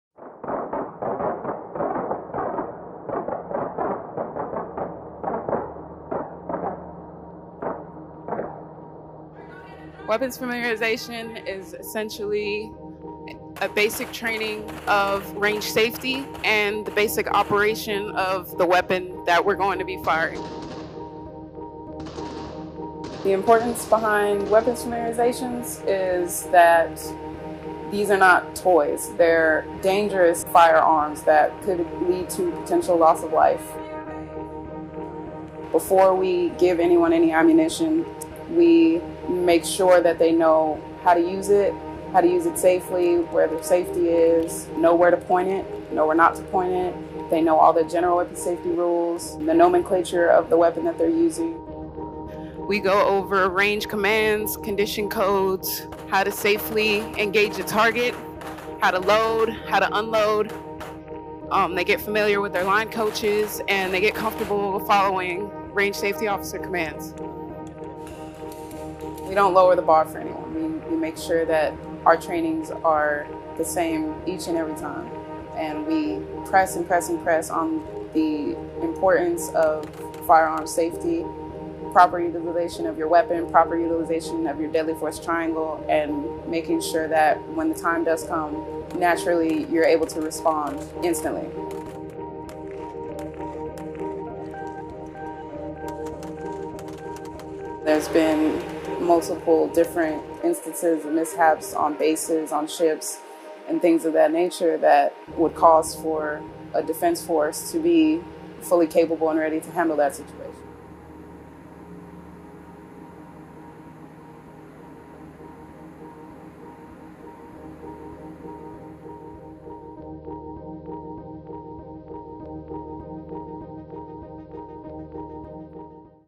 USS Bataan Sailors Participate in a Gun Qualification Evolution.
USS Bataan Sailors Participate in Gun Qualification Evolution
UNITED STATES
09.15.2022
Video by Seaman Christopher Jones
USS Bataan (LHD 5)